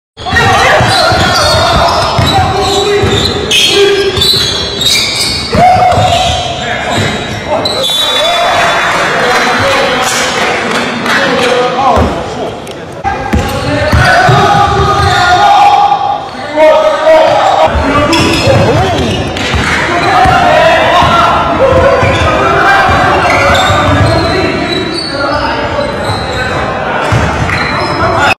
[sina.com]麦迪闪耀队内训练赛 抢断送助攻 穿人缝妙传 121025
Tracy McGrady、T-Mac、麦迪、麦蒂、China、中国、CBA、Qingdao、青岛双星雄鹰男篮、全队训练、Team training、新浪、sina.com